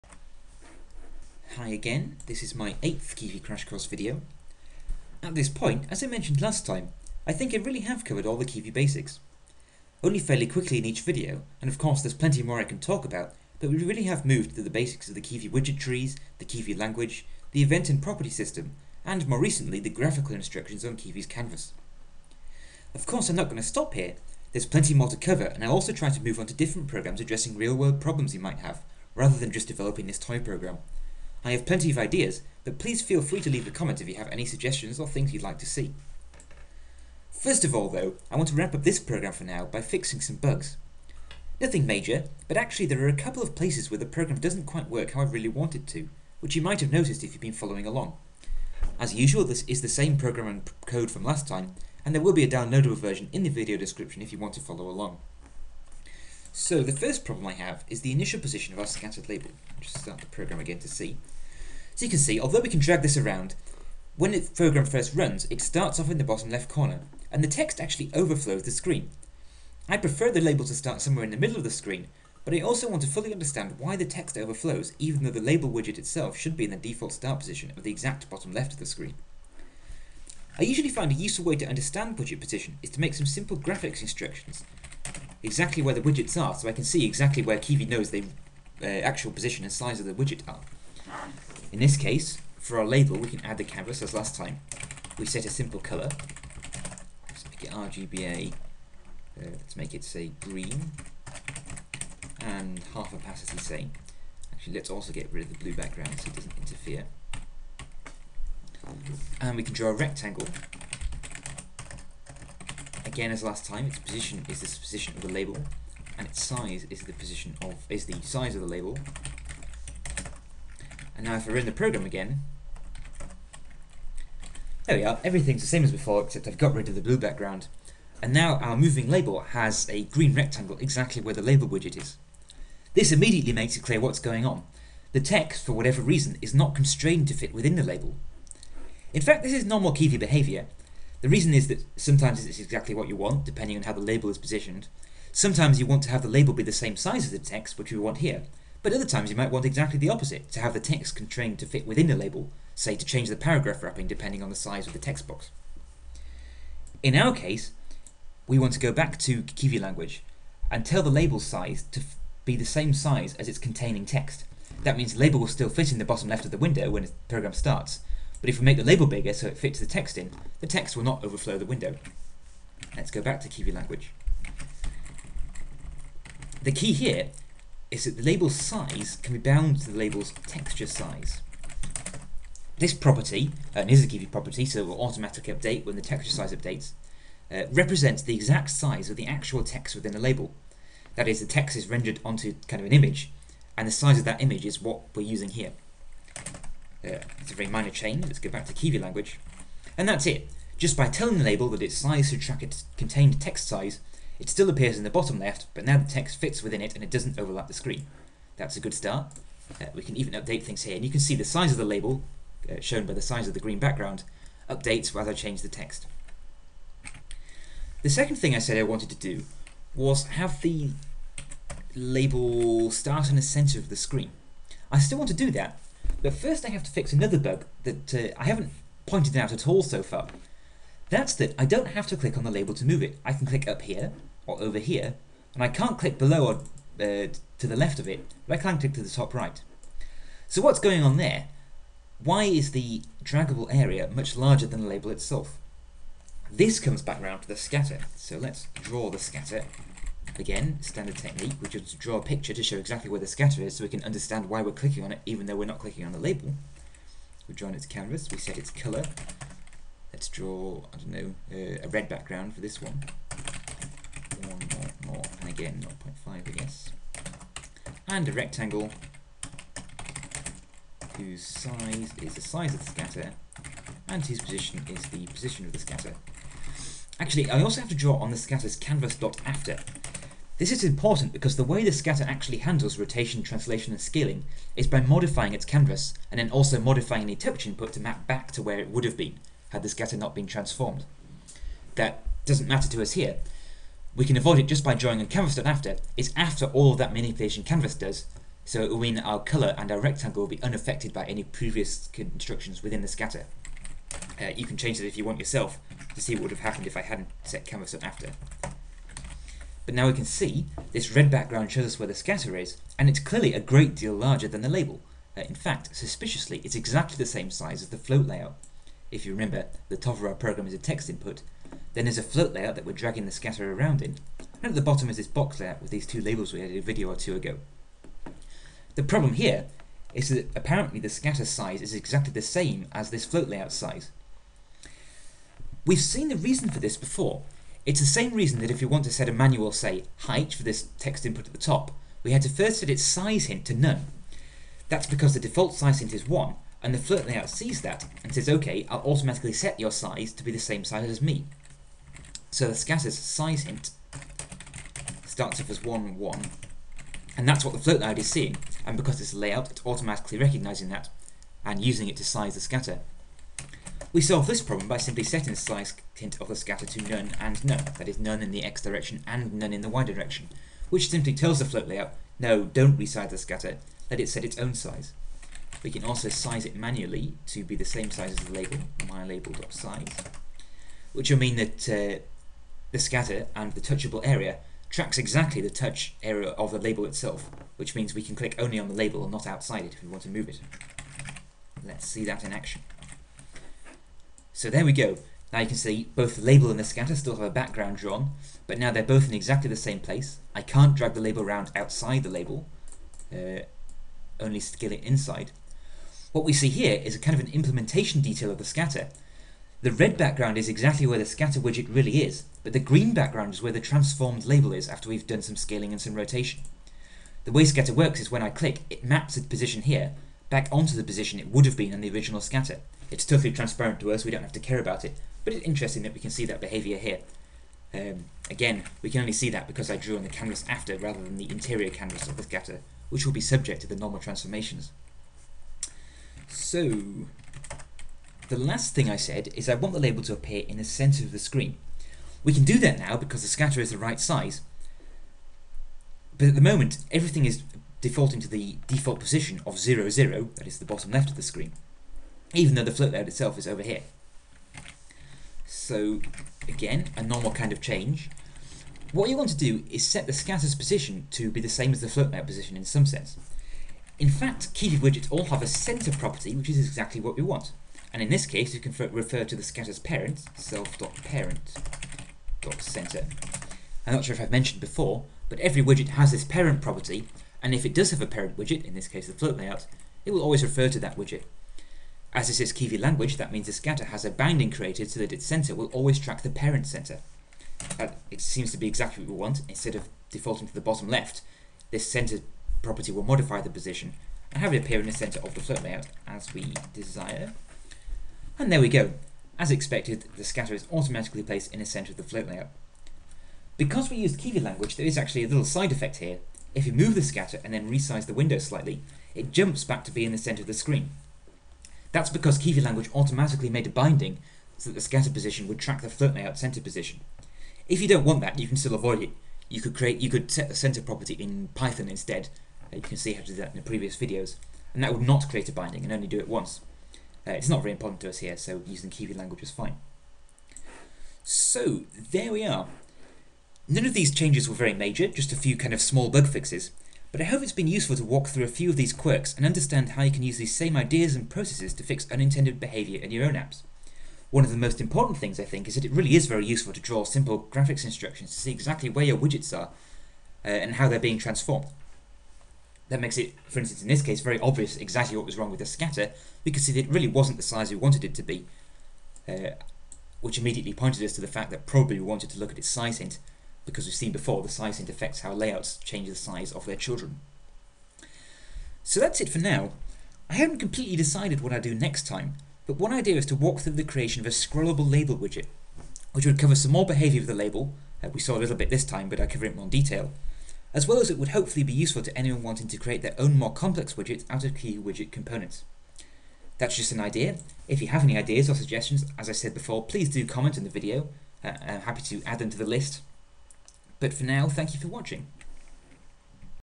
Kivy crash course 8: Let's fix some bugs
In this video I set out to fix a few bugs and weird behaviours in our toy program, mostly to do with some default widget sizes and positions that lead to unexpected behaviour like the ability to move the label by clicking nowhere near it.

I think this video will mostly round off this toy program, and intend to move to cover some different things in future videos, though I might come back to this one to cover more basics like animation or manual touch handling.